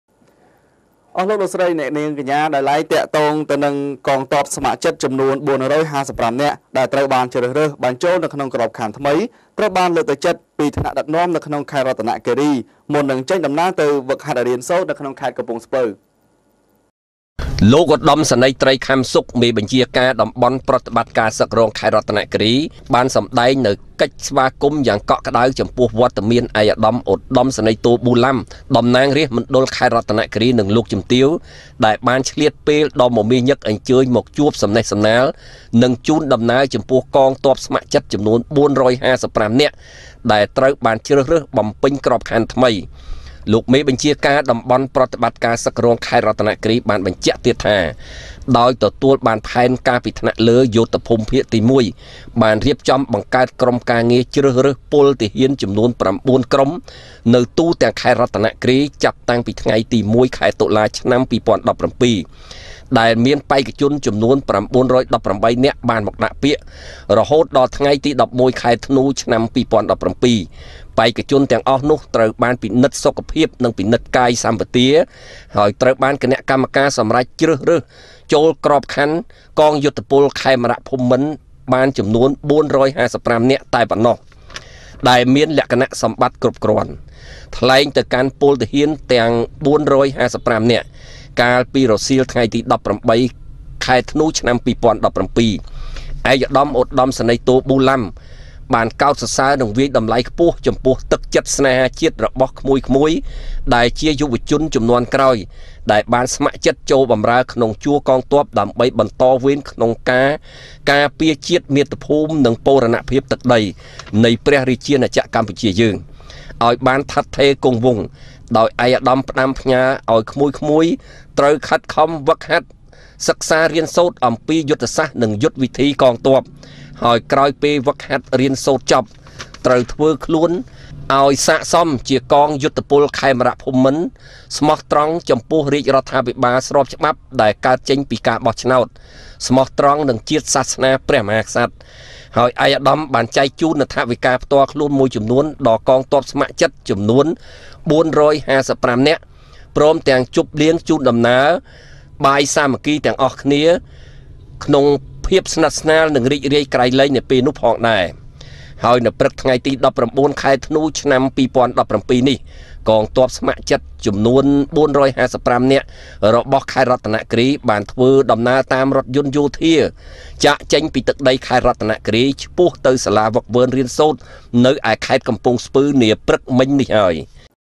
កងទ័ពស្ម័គ្រចិត្តចំនួន៤៥៥នាក់ ដែលត្រូវបានជ្រើសរើសបញ្ចូលក្របខ័ណ្ឌថ្មី
កងទ័ពស្ម័គ្រចិត្តចំនួន៤៥៥នាក់ ដែលត្រូវបានជ្រើសរើសបញ្ចូលក្របខ័ណ្ឌថ្មី ត្រូវបានលើកទឹកចិត្តពីថ្នាក់ដឹកនាំក្នុងខេត្តរតនគិរី មុននឹងចេញដំណើរទៅហ្វឹកហ្វឺនរៀនសូត្រ នៅខេត្តកំពង់ស្ពឺ